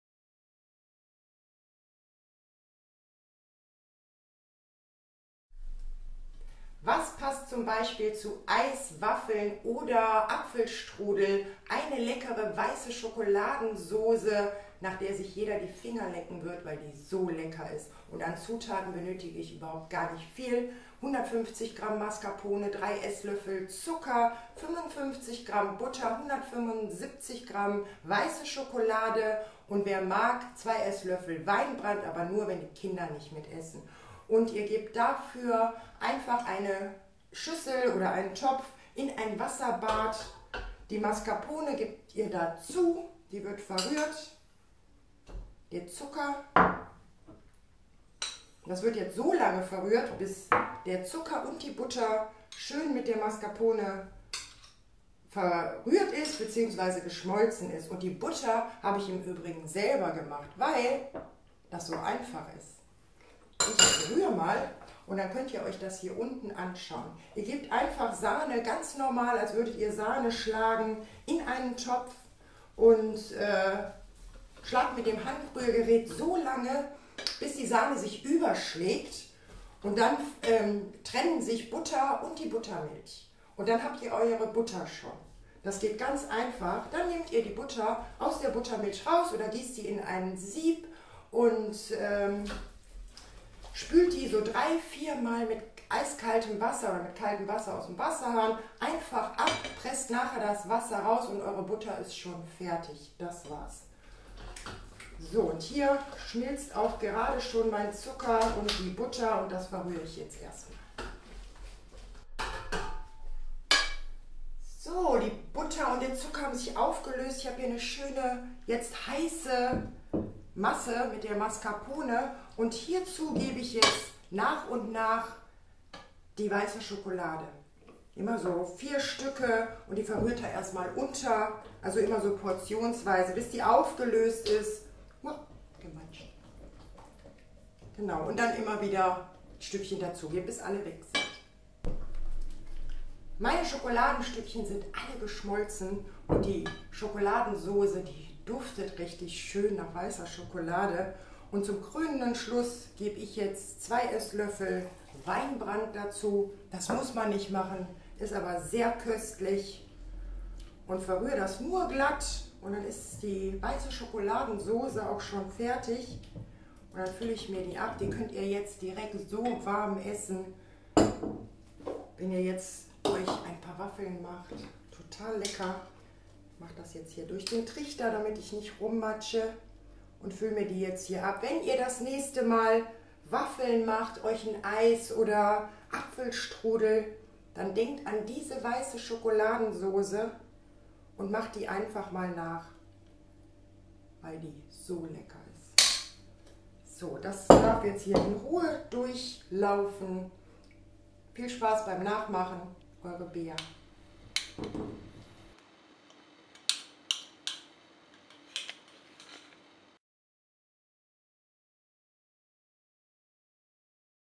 SCHOKOLADENSAUCE Rezept - Die beste helle Dessertsoße selber machen
Weiße Schokoladensauce – Dessertsoße selber machen. Leckere Dessertsoßen sind so schnell und einfach selber gemacht. Mit wenigen Zutaten die eigentlich immer im Haus sind kann man herrliche Soßen zaubern und mit Aromen verfeinern.

Bei meinem Rezept für Weiße Schokoladensoße ist die Soße mit Weinbrand verfeinert, das schmeckt richtig lecker und passt zu vielen leckeren Sachen wie .z.B.

Für meine Schokoladensoße habe ich auch schnell die Butter selber gemacht, wie das geht seht ihr im Video. Das ist so Kinderleicht gemacht, genauso wie die Soße. Also wenn ihr das nächste Mal eine süße weiße Schokoladensoße machen möchtet, kann ich euch diese Variante nur ans Herz legen.

Ach ja und wenn Kinder mit schlemmen, bitte den Weinbrand weg lassen, dafür könnte man die Soße mit Orangenschalen Abrieb verfeinern oder mit Orangen Aroma, auch lecker :-)